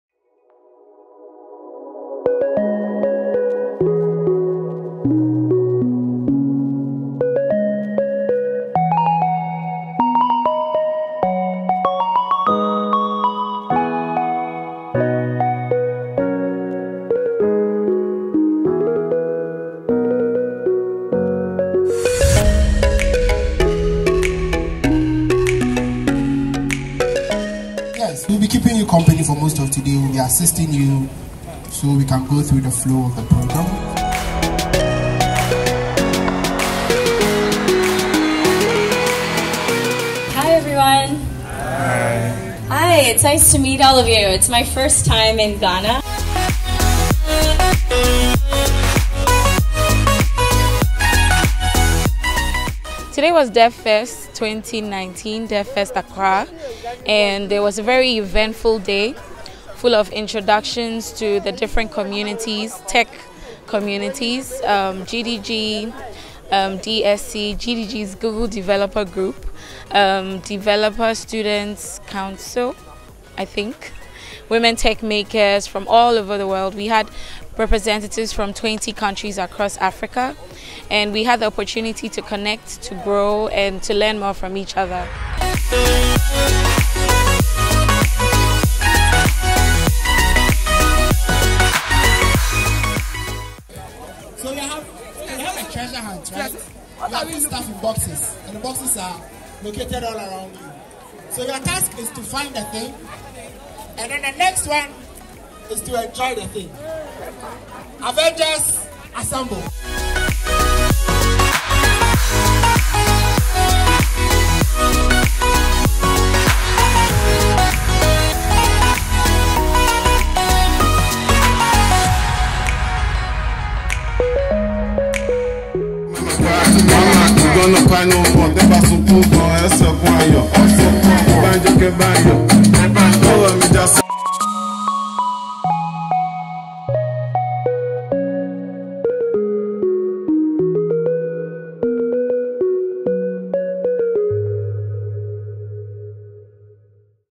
GDG Accra DevFest 2019 – Shot with Huawei P30 Lite and Movi Robot
"DevFests are community-led developer events hosted by Google Developer Groups around the globe. GDGs are focused on community building and learning about Google’s technologies. "

"DevFests may cover multiple products such as Android, Firebase, Google Cloud Platform, Google Assistant, Flutter, machine learning with TensorFlow, and mobile web. Events may include speaker sessions, hackathons, codelabs, study jams, and trainings with speakers from Google, Google Developers Experts, Women Techmaker Ambassadors and speakers from other companies."

This year, GDG Accra hosted its DevFest which brought participants from about 20 different African countries together. This year’s theme was Build, Collaborate, and Scale.